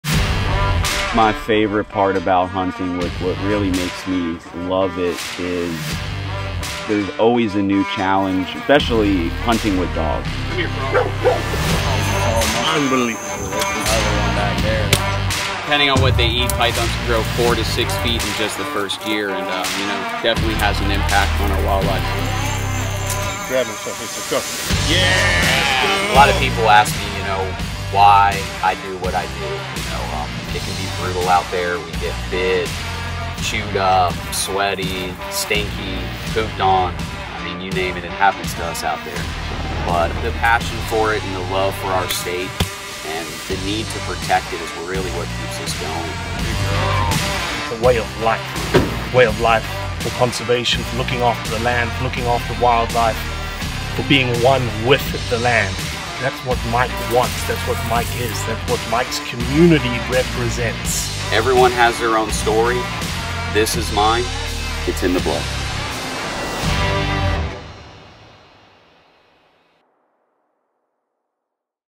Python Cowboy
Iguana and Python management is a tedious, difficult and necessary management tool for sustaining Florida’s ecological system.

Hunting is a crucial tool for wildlife management in The Everglades.